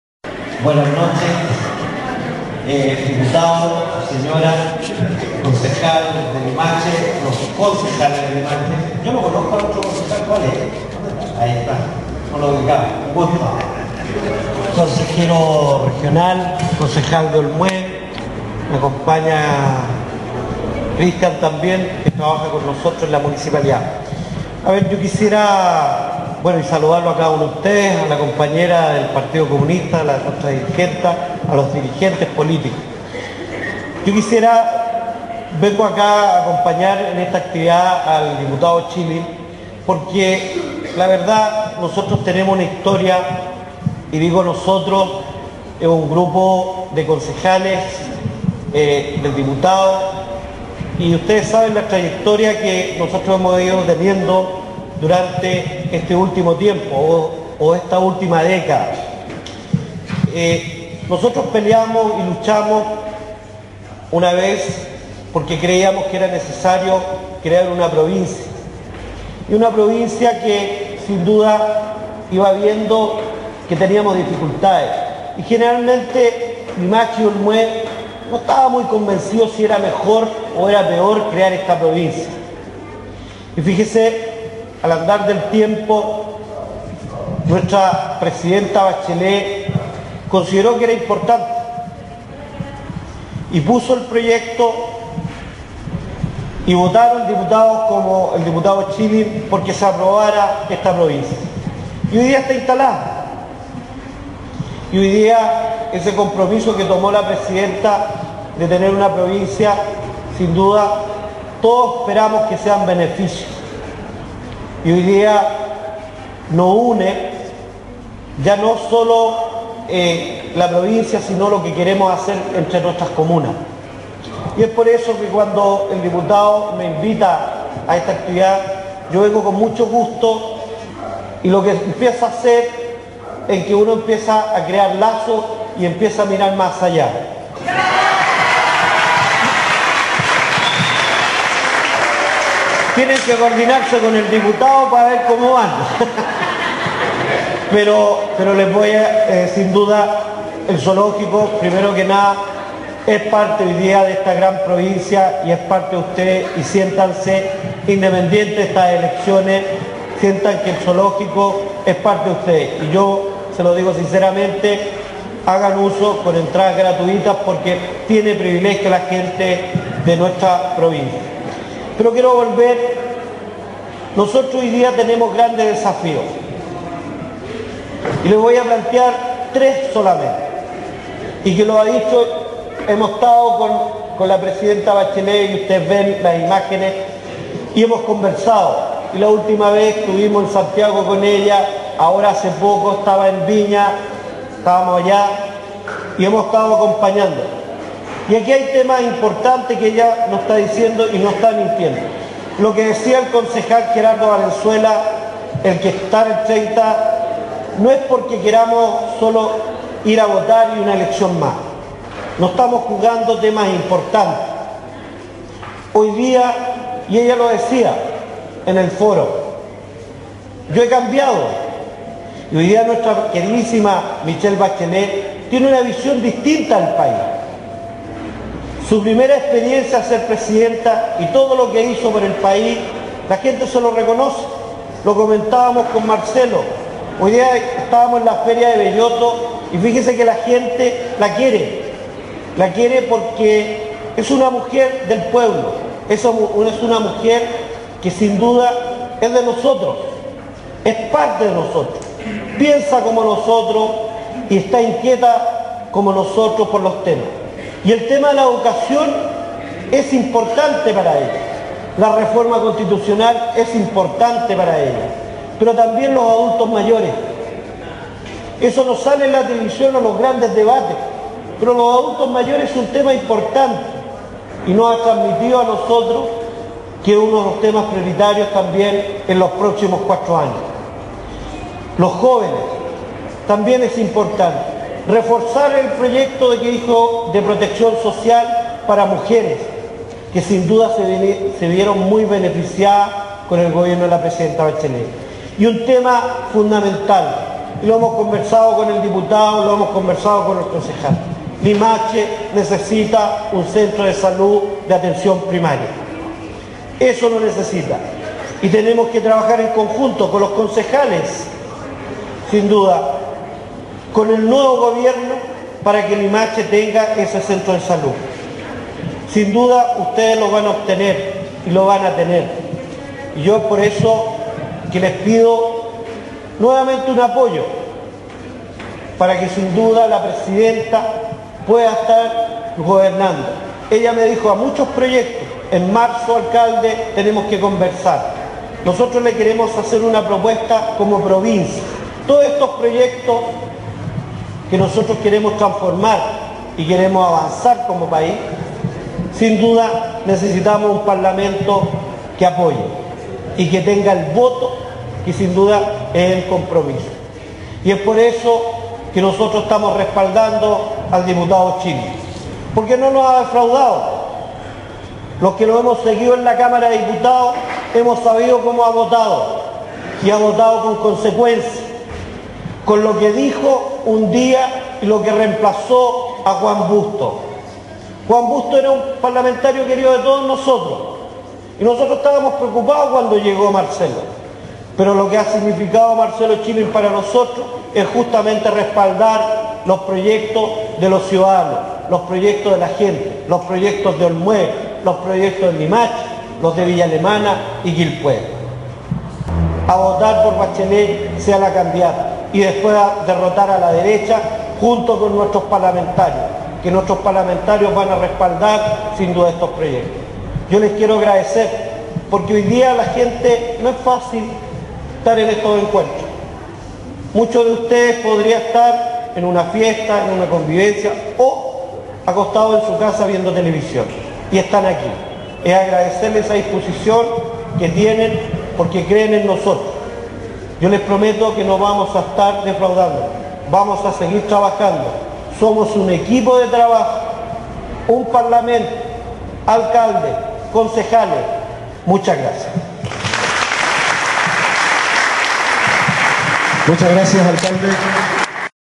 filtran audio de apoyo de Viñambres a Schilling
el alcalde de Quilpué finalmente sacó la voz para definir su postura por Marcelo Schlling. esto se debería ante la aplastante evidencia de la realidad y los numerosos sondeos realizados.